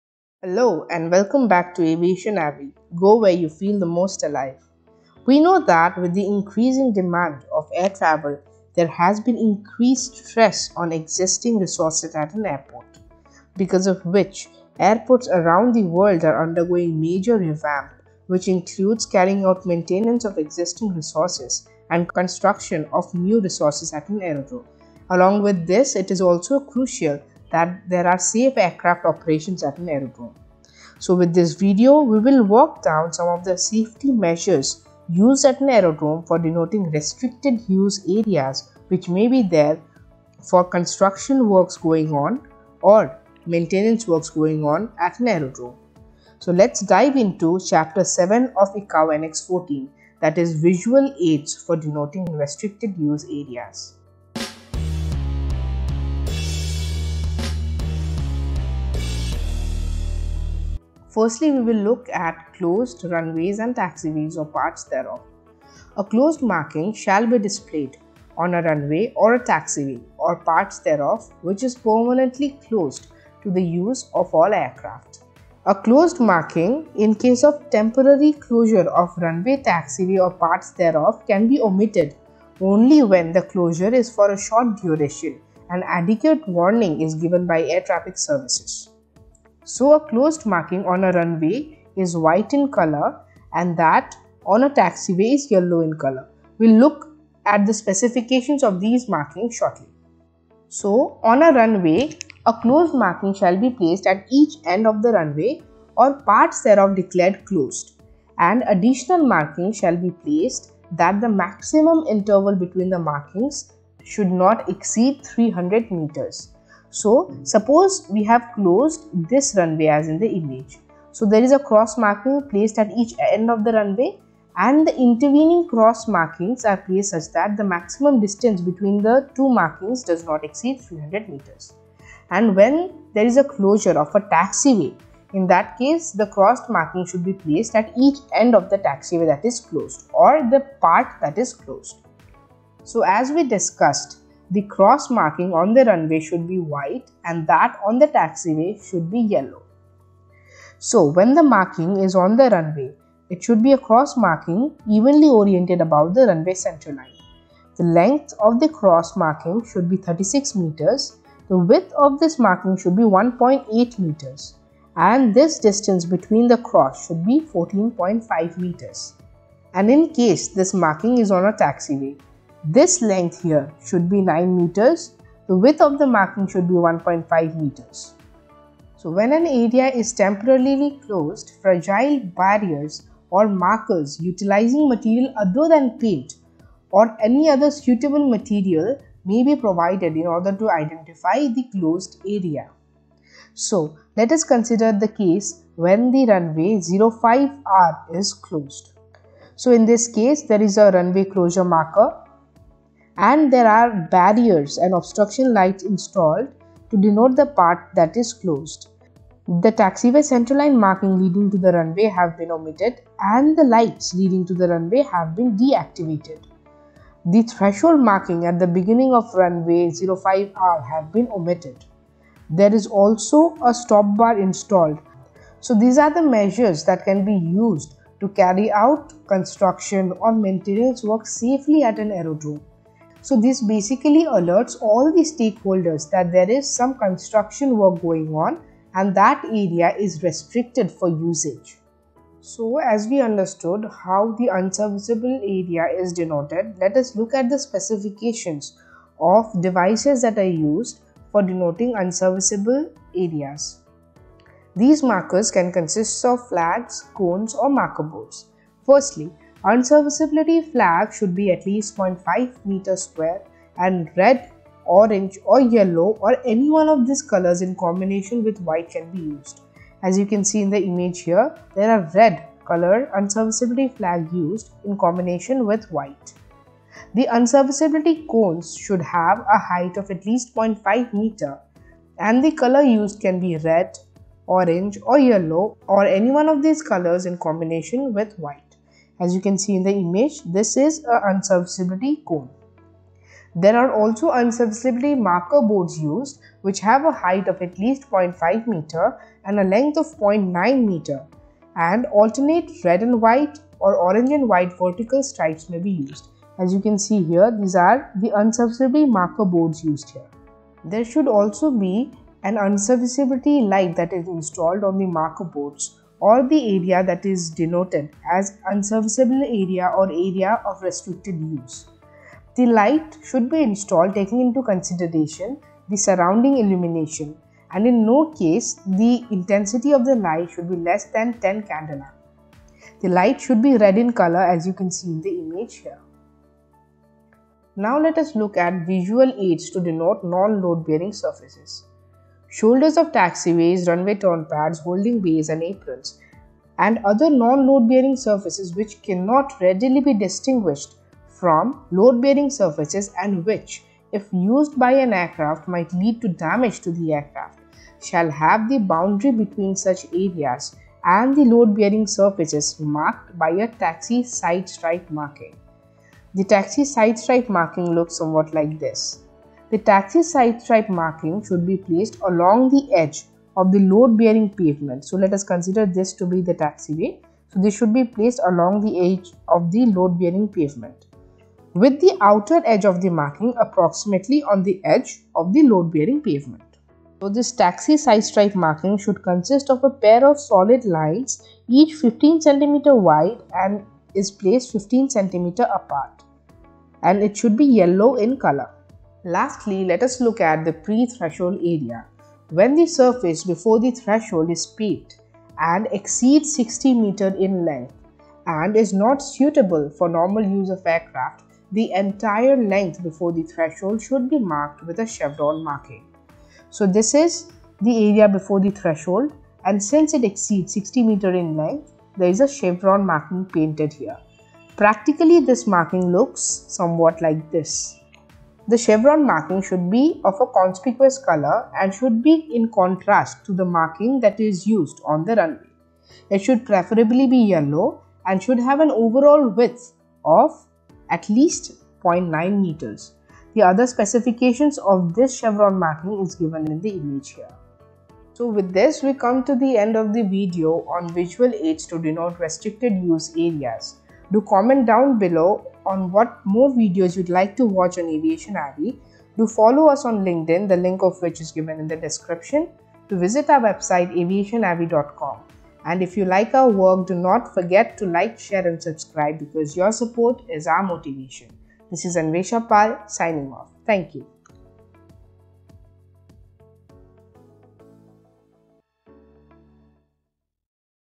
Visual Aids to Denote Restricted Use Areas- Chapter 7, ICAO Annex14
This video will help you understand Visual Aids to Denote Restricted Use Areas with the help of ICAO Annex 14 Chapter 7.

Lets meet up on :

AviationAvi.com

Go where you feel the most alive!